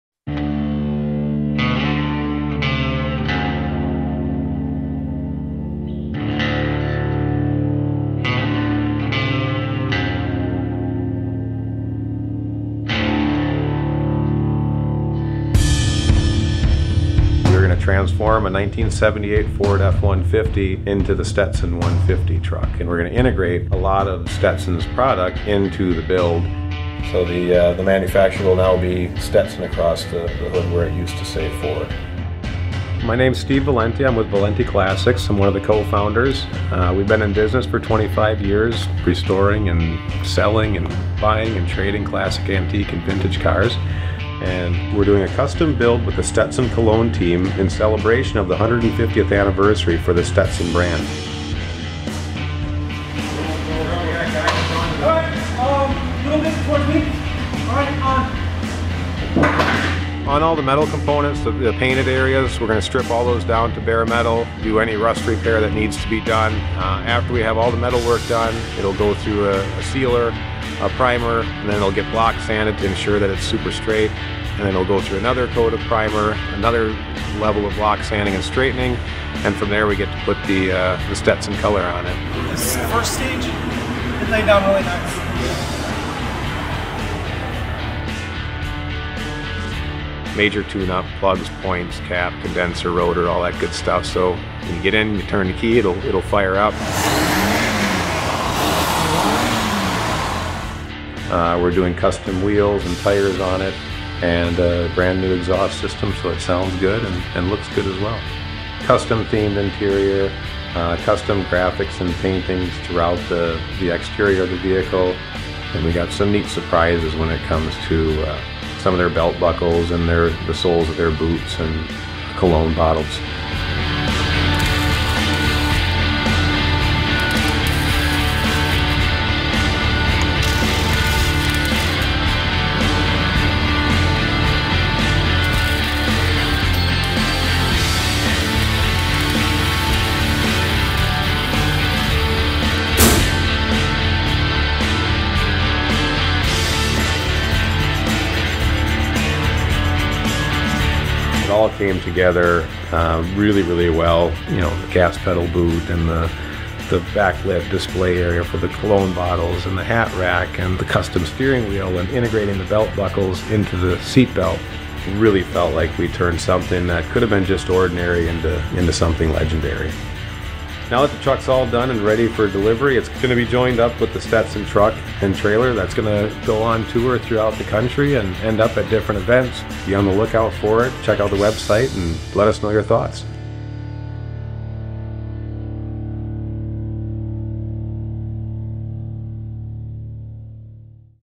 Stetson150 - A Legendary Truck
Where the leather meets the road: Making a 150 year old brand relevant for a new generation.

Stetson celebrated 150 years of being a legendary, iconic American brand. But when you’re 150 years old, sometimes your image is a little worn and dusty. (Perfect for a brand like Stetson.) So we saddled up and helped Stetson Cologne celebrate the anniversary by turning to another legendary 150: a vintage Ford F150 pick-up, which we transformed into the S150. And we made it totally badass, a blend of classic and contemporary, with details and surprises that attracted cool guys and even their dads and uncles. It became the perfect vehicle for a national sampling program. And reminding you that your dad was actually cool. Who knew?